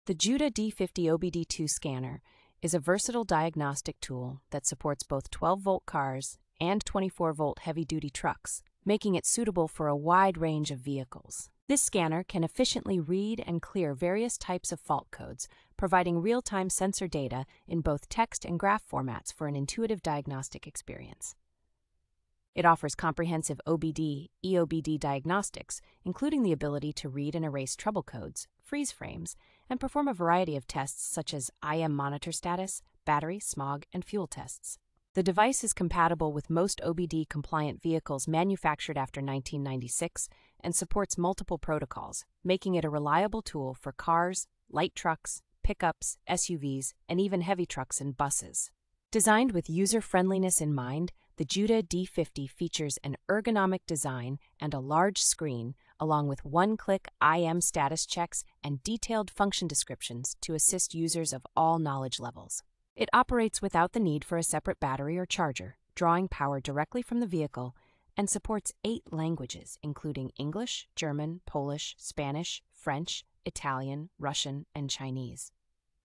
REVIEW (2025): JUTA D50 OBD2 Scanner. ESSENTIAL details.
We're highlighting the main attractions of the JUTA D50 OBD2 Scanner in this short overview.

The JUTA D50 OBD2 Scanner is a versatile diagnostic tool that supports both twelve volt cars and twenty-four volt heavy-duty trucks, making it suitable for a wide range of vehicles.

This scanner can efficiently read and clear various types of fault codes, providing real-time sensor data in both text and graph formats for an intuitive diagnostic experience.

It offers comprehensive OBD/EOBD diagnostics, including the ability to read and erase trouble codes, freeze frames, and perform a variety of tests such as I/M monitor status, battery, smog, and fuel tests.

The device is compatible with most OBDII-compliant vehicles manufactured after nineteen ninety-six and supports multiple protocols, making it a reliable tool for cars, light trucks, pickups, SUVs, and even heavy trucks and buses.

Designed with user-friendliness in mind, the JUTA D50 features an ergonomic design and a large screen, along with one-click I/M status checks and detailed function descriptions to assist users of all knowledge levels.

It operates without the need for a separate battery or charger, drawing power directly from the vehicle, and supports eight languages including English, German, Polish, Spanish, French, Italian, Russian, and Chinese.